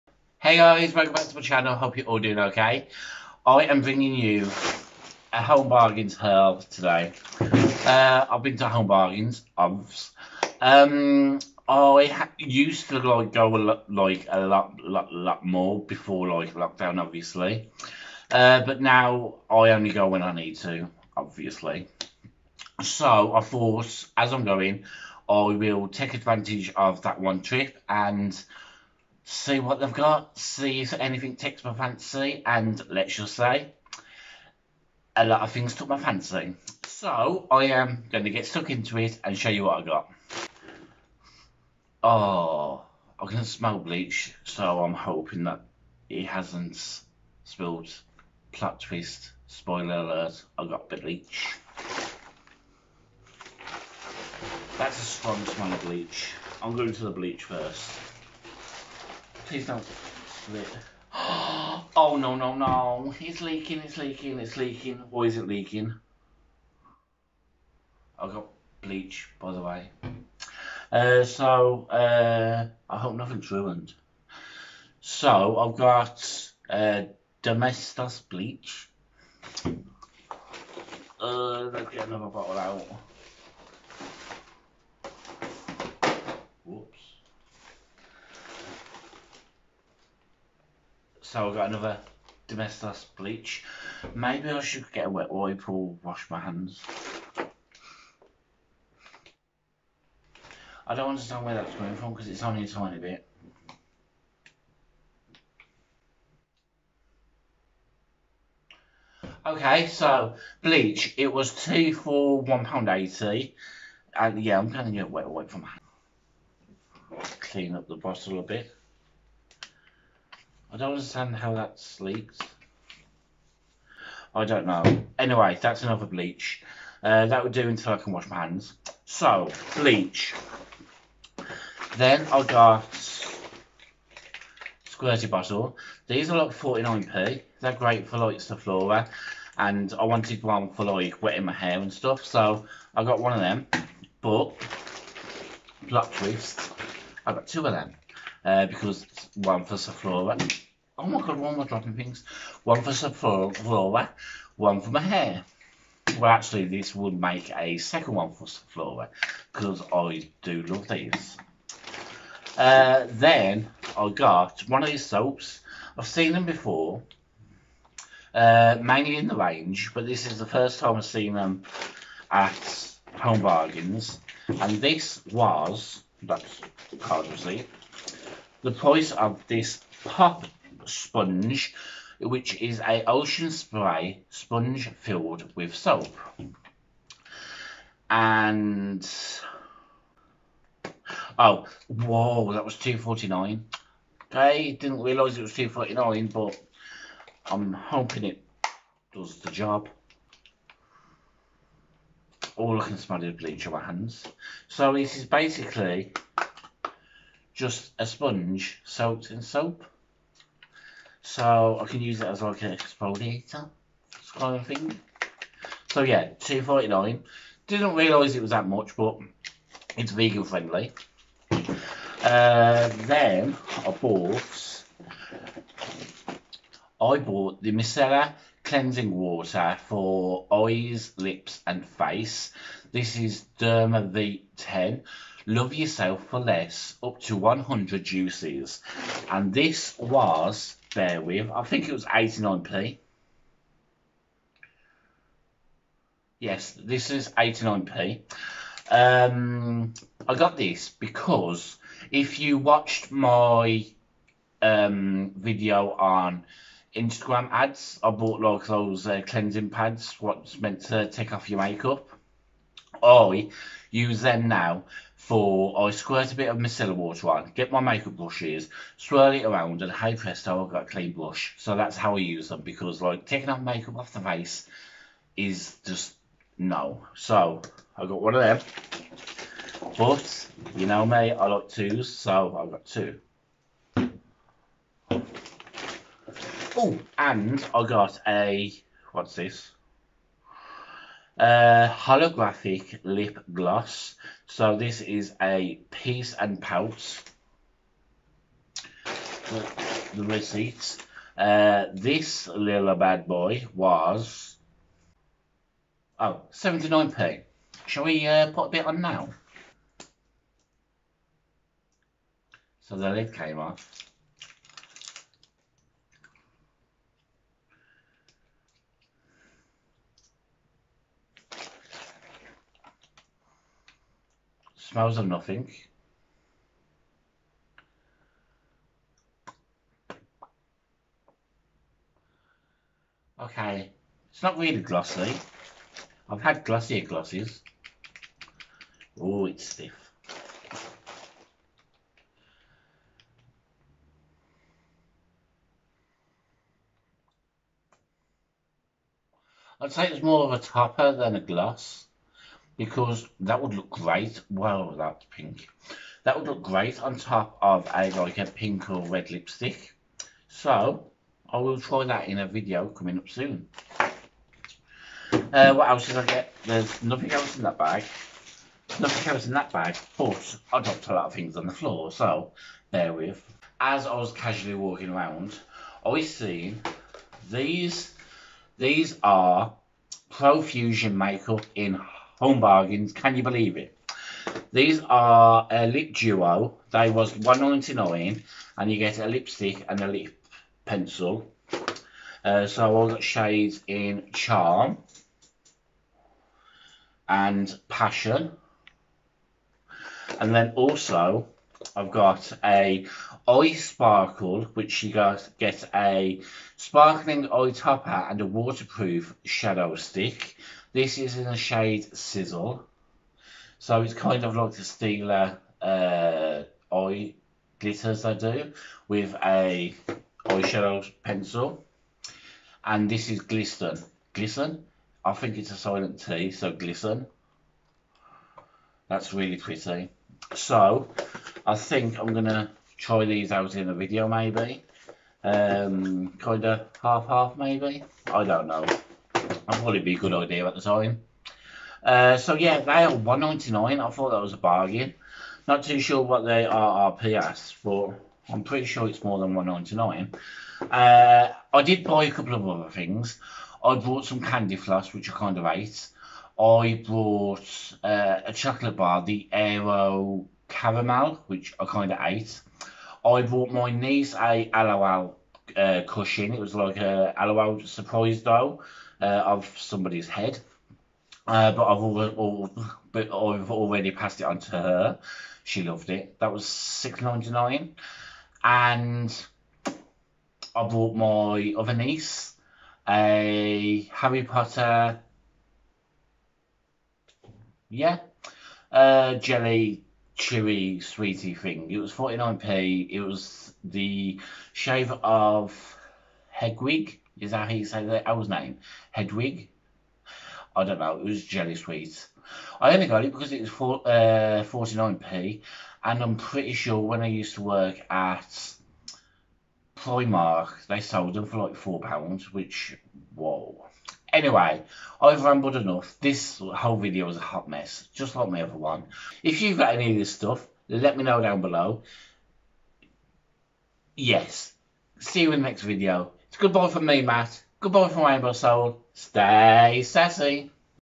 A HOT MESS OF A HAUL cleanup in aisle living room
So I went to Home Bargains and this is what I got